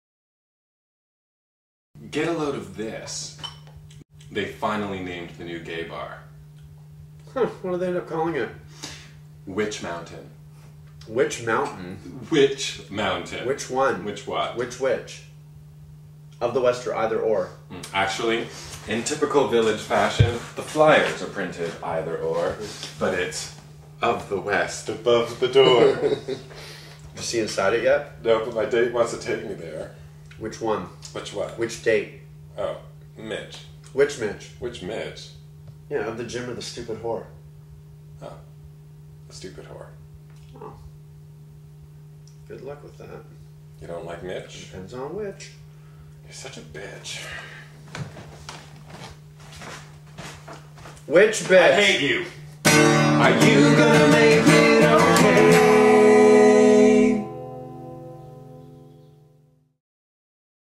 Cordelia 1.3 - Which?
There is a camera in David and Ryan's new television. They do not know this. Today, David has BIG news.